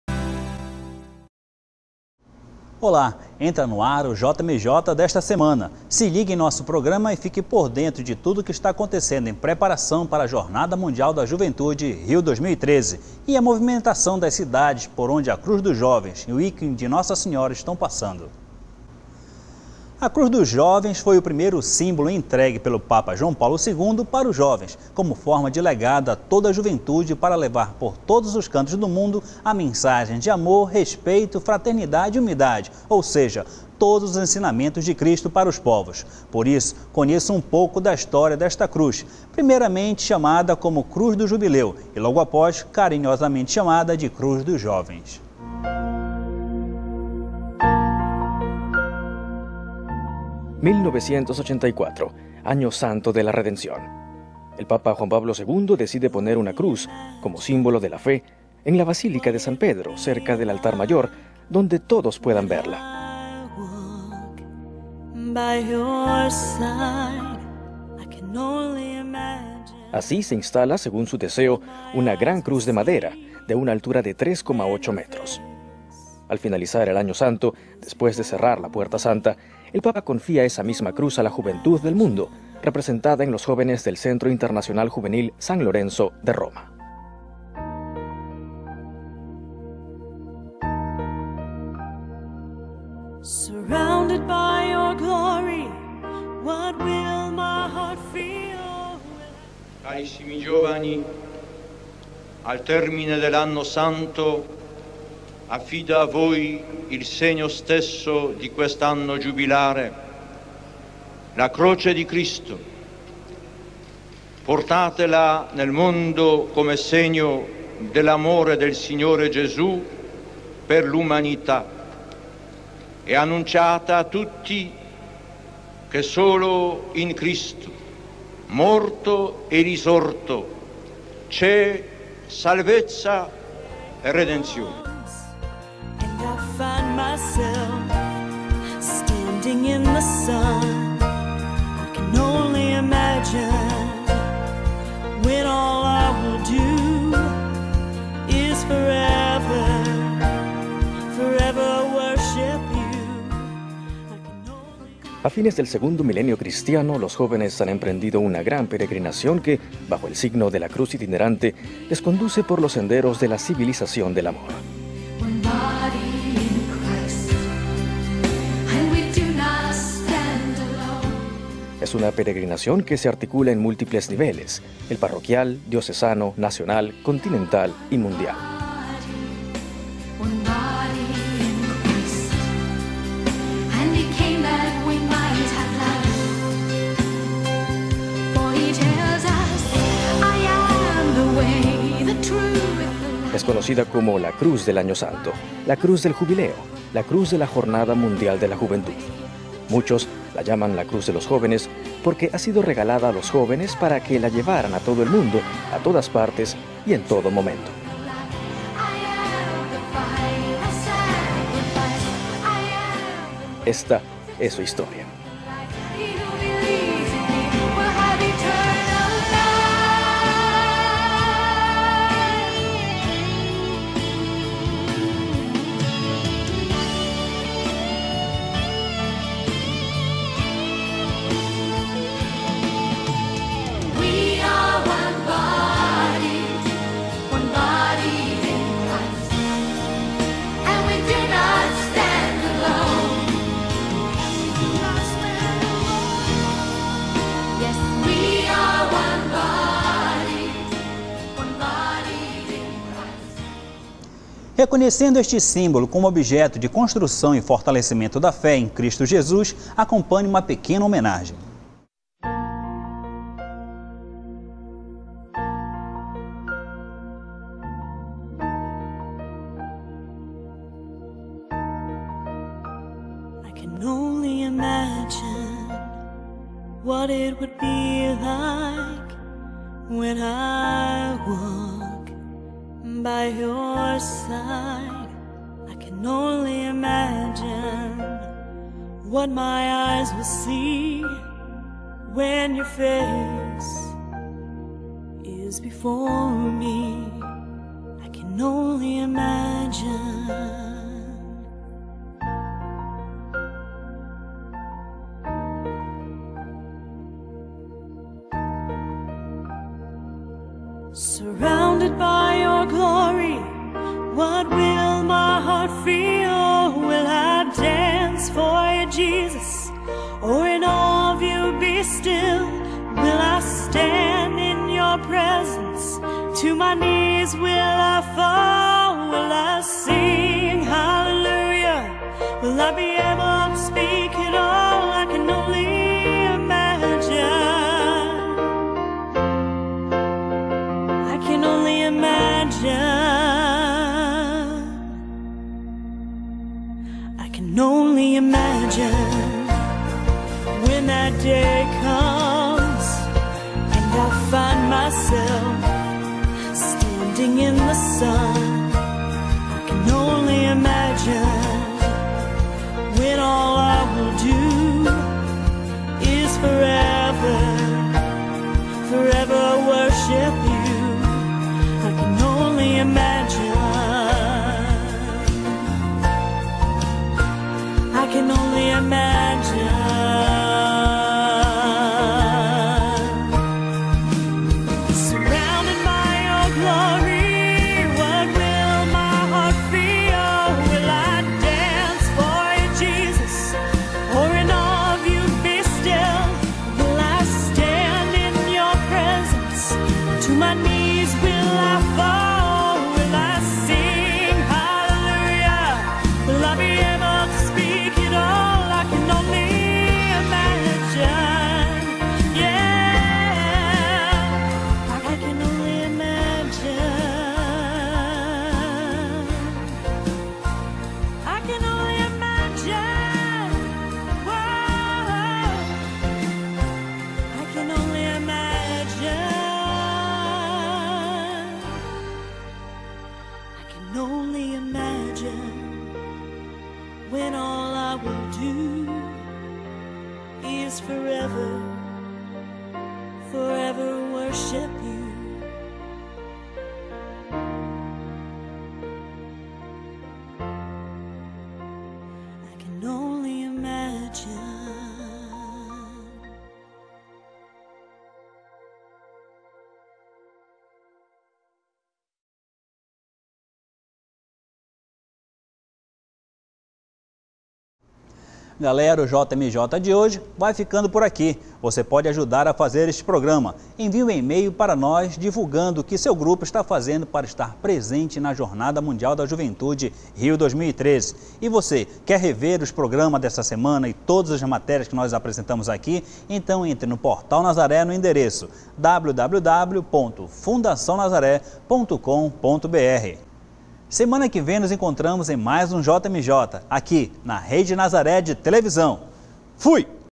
JMJ - 11/08/2012
Aqui você fica por dentro da programação da Jornada Mundial da Juventude 2013 e da passagem do ícone de Nossa Senhora e da Cruz da jornada pelos estados brasileiros. Apresentação: Sidnney Mendes. TV Nazaré, Belém-PA.